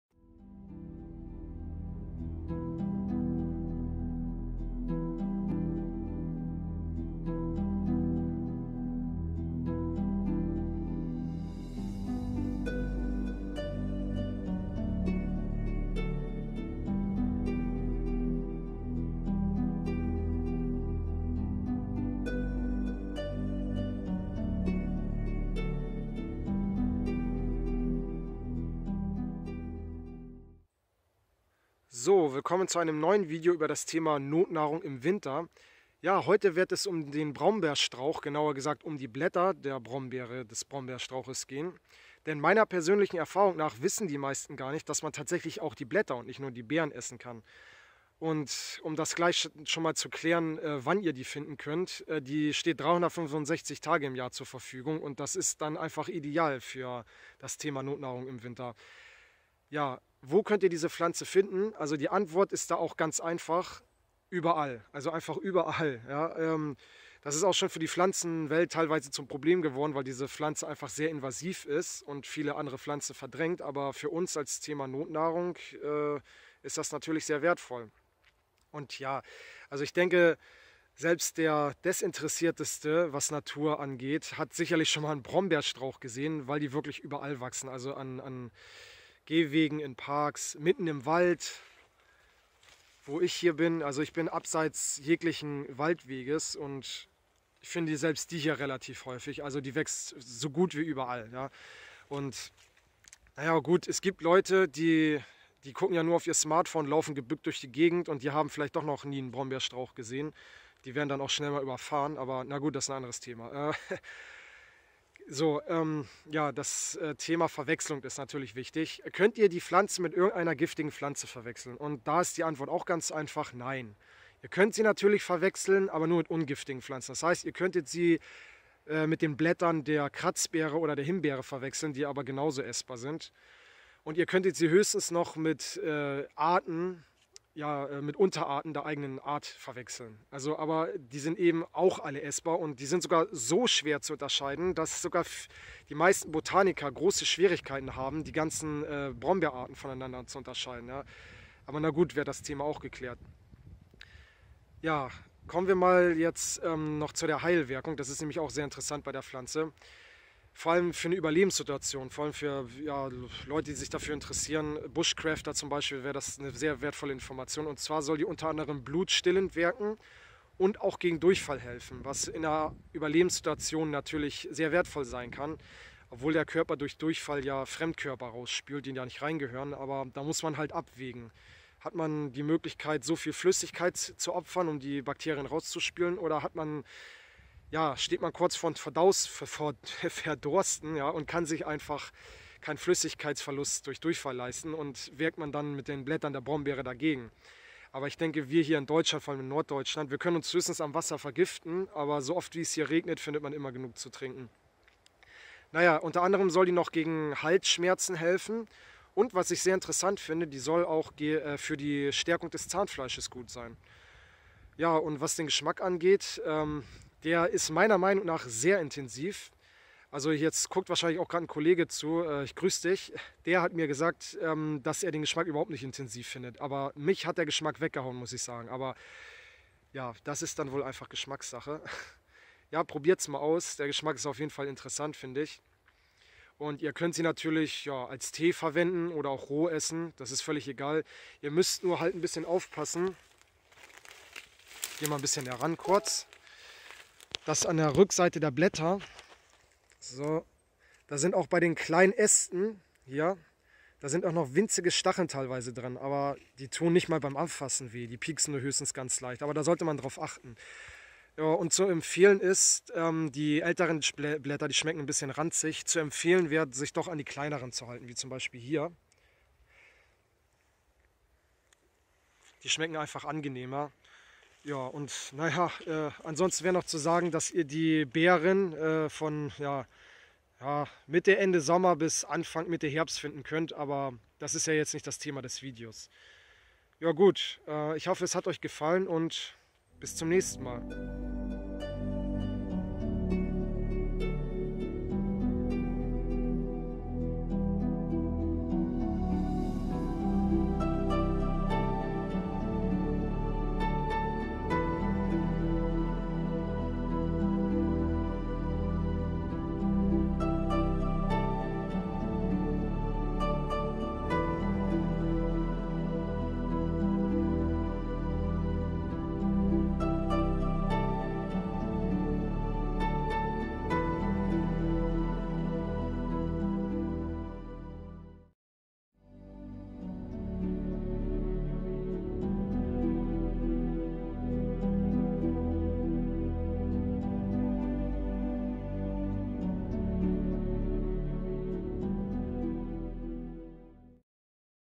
Notnahrung im Winter ▶ Die Brombeere (Blätter)
Weitere interessante Anwendungsgebiete:

Wegen der schleimlösenden Eigenschaften kann man Brombeerblätter auch gegen Verschleimung im Bereich der Atmungsorgane verwenden.

Die blutreinigende Wirkung führt zu einer Verbesserung von Hautproblemen wie Ekzemen und schlecht heilenden Wunden.

Vor der Geburt eignet sich Brombeerblätter-Tee zur Auflockerung des Gewebes, damit die Geburt später leicht vonstatten geht.